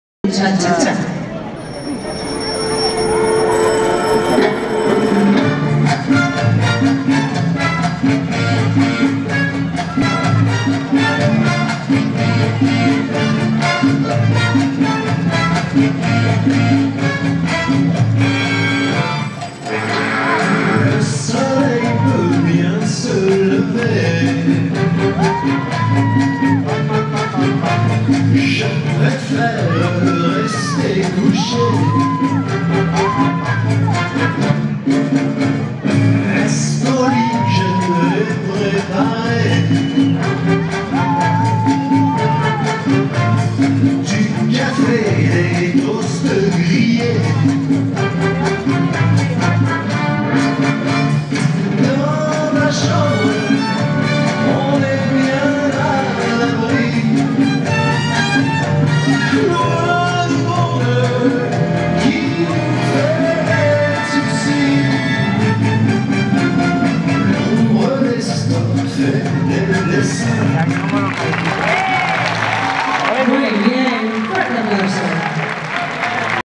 GUILLE Y PILAR: Chachacha.
Final Campeonato España 20 Octubre 2013: Cataluña versus Valencia. Gana Valencia.